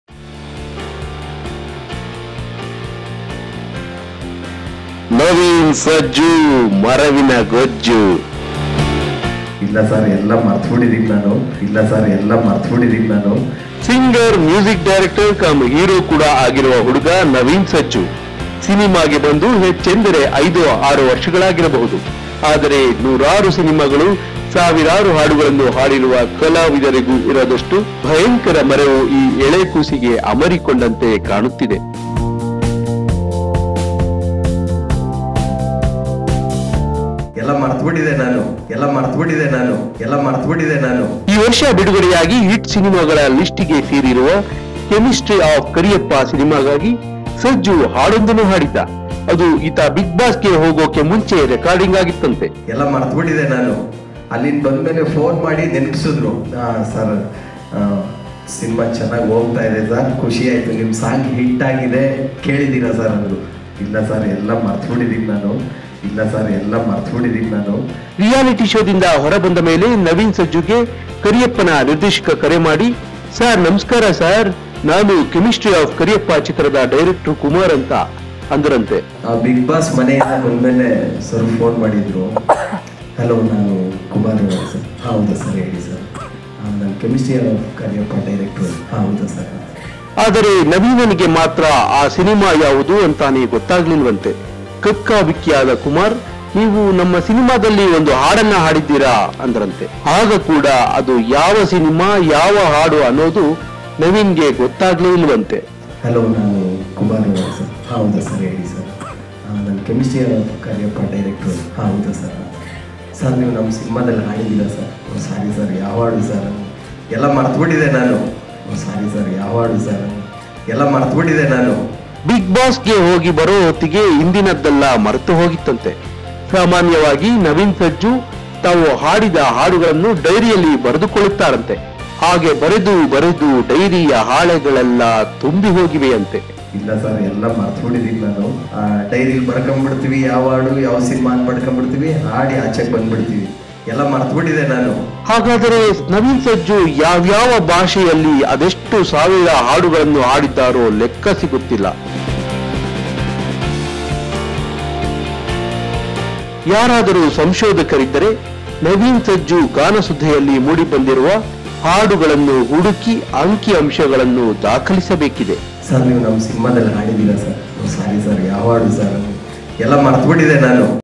ನವೀನ್ ಸಜ್ಜು ಮರೆವಿನ ಗೊಜ್ಜು!
ಈ ವರ್ಷ ಬಿಡುಗಡೆಯಾಗಿ ಹಿಟ್ ಸಿನಿಮಾ ಲಿಸ್ಟಿಗೆ ಸೇರಿರುವ ಕೆಮಿಸ್ಟ್ರಿ ಅಫ್ ಕರಿಯಪ್ಪ ಸಿನಿಮಾಗಾಗಿ ಸಜ್ಜು ಹಾಡೊಂದನ್ನು ಹಾಡಿದ್ದ. ಅದು ಈತ ಬಿಗ್ ಬಾಸ್ ಗೆ ಹೋಗೋಕೆ ಮುಂಚೆ ರೆಕಾರ್ಡಿಂಗ್ ಆಗಿತ್ತಂತೆ. ರಿಯಾಲಿಟಿ ಶೋದಿಂದ ಹೊರಬಂದಮೇಲೆ ನವೀನ್ ಸಜ್ಜುಗೆ ಕರಿಯಪ್ಪನ ನಿರ್ದೇಶಕ ಕರೆ ಮಾಡಿ ‘ಸರ್ ನಮಸ್ಕಾರ ಸರ್… ನಾನು ಕೆಮಿಸ್ಟ್ರಿ ಅಫ್ ಕರಿಯಪ್ಪ ಸಿನಿಮಾ ಡೈರೆಕ್ಟ್ರು ಕುಮಾರ್ ಅಂತಾ…’ ಅಂದರಂತೆ. ಅದರೆ ನವೀನನಿಗೆ ಮಾತ್ರ ಆ ಸಿನಿಮಾ ಯವುದು ಅಂತಾನೇ ಗೊತ್ತಾಗಲಿಲ್ಲವಂತೆ. ಕಕ್ಕಾಬಿಕ್ಕಿಯಾದ ಕುಮಾರ್ ‘ನೀವು ನಮ್ಮ ಸಿನಿಮಾದಲ್ಲಿ ಒಂದು ಹಾಡು ಹಾಡಿದ್ದೀರ’ ಎಂದರಂತೆ. ಆಗ ಕೂಡಾ ಅದು ಯಾವ ಸಿನಿಮಾ, ಯಾವ ಹಾಡು ಅನ್ನೋದು ಗೊತ್ತಾಗಲಿಲ್ಲವಂತೆ‌. ಬಿಗ್ ಬಾಸ್ ಗೆ ಹೋಗಿ ಬರೋ ಹೊತ್ತಿಗೆ ಹಿಂದಿನದ್ದೆಲ್ಲಾ ಮರೆತೋಗಿತ್ತಂತೆ. ಸಾಮಾನ್ಯವಾಗಿ ನವೀನ್ ಸಜ್ಜು ತಾವು ಹಾಡಿದ ಹಾಡುಗಳನ್ನು ಡೈರಿಯಲ್ಲಿ ಬರೆದುಕೊಳ್ಳುತ್ತಾರಂತೆ. ಹಾಗೆ ಬರೆದೂ ಬರೆದೂ ಡೈರಿಯ ಹಾಳೆಗಳೆಲ್ಲಾ ತುಂಬಿಹೋಗಿವೆಯಂತೆ.